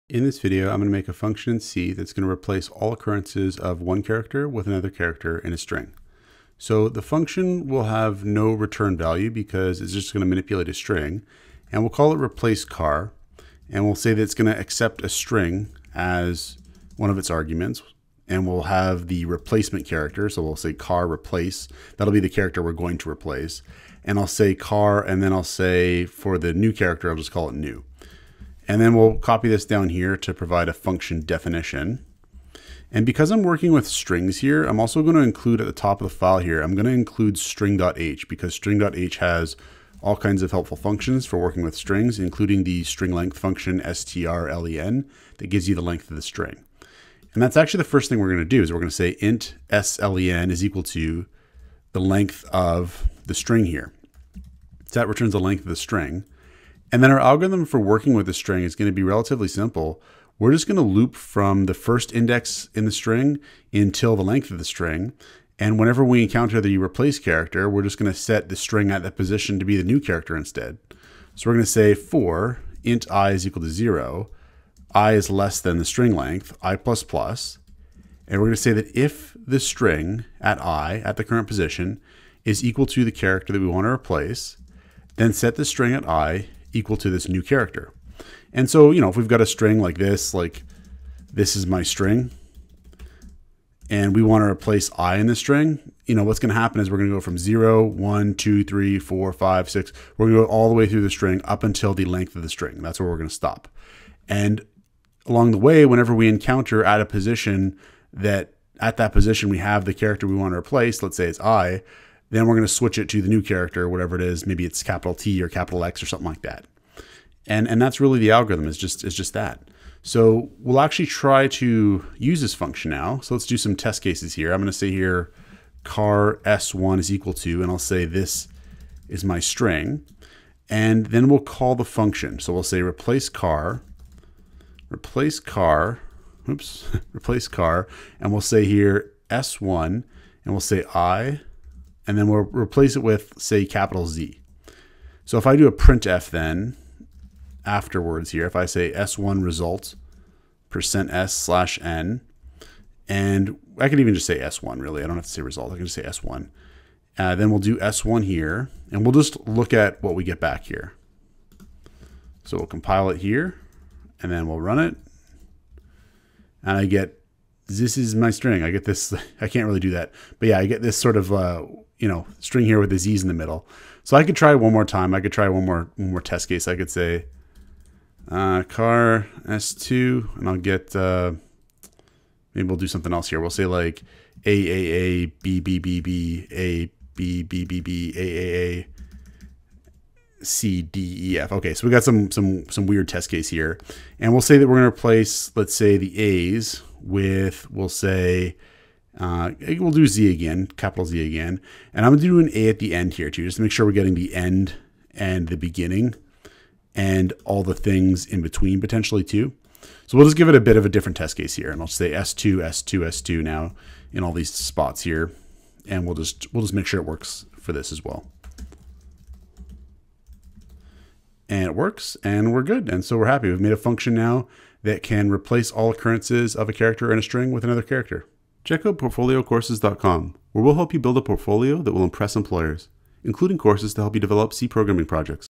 Replace a character in a string with another character | C Programming Example
An example of a function to replace all occurrences of a character in a string with another character using C. Source code to build a portfolio that will impress employers!